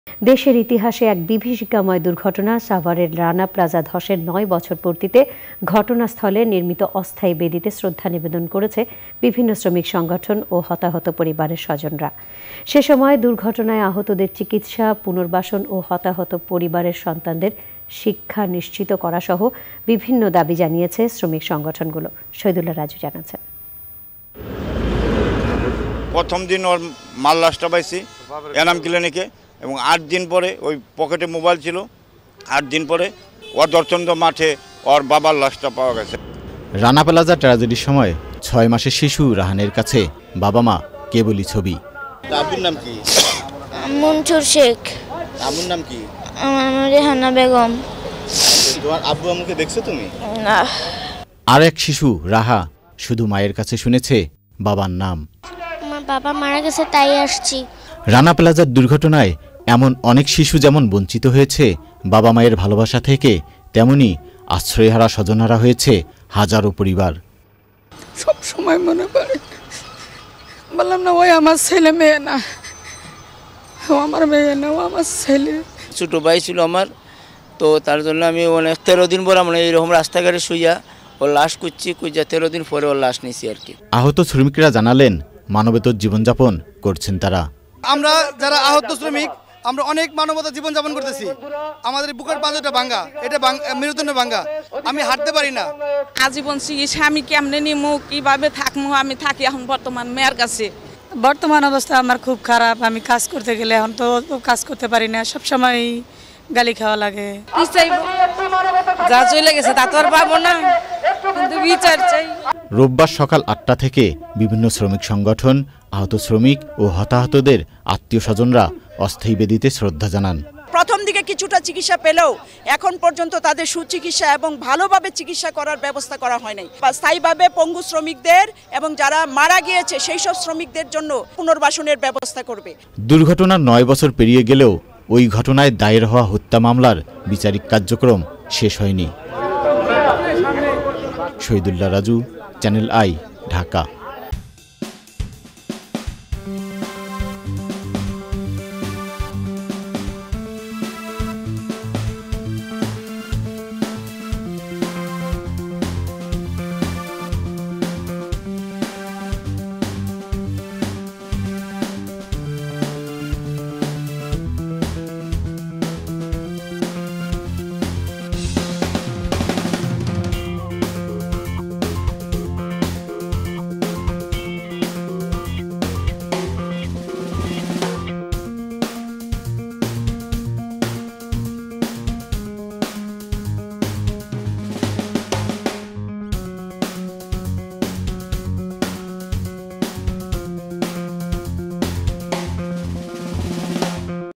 রানা প্লাজা ট্র্যাজেডির ৯ বছরে শ্রদ্ধা-ভালোবাসায় নিহত শ্রমিকদের স্মরণ
দেশের ইতিহাসে এক বিভীষিকাময় দুর্ঘটনা সাভারের রানা প্লাজা ধসের ৯ বছর পূর্তিতে ঘটনাস্থলে নির্মিত অস্থায়ী বেদিতে শ্রদ্ধা নিবেদন করেছে বিভিন্ন শ্রমিক সংগঠন ও হতাহত পরিবারের স্বজনরা। সে সময় দুর্ঘটনায় আহতদের চিকিৎসা, পুনর্বাসন ও হতাহত পরিবারের সন্তানদের শিক্ষা নিশ্চিত করাসহ বিভিন্ন দাবি জানিয়েছে শ্রমিক সংগঠনগুলো। শহীদুল্লাহ রাজুর রিপোর্ট।

worldwide.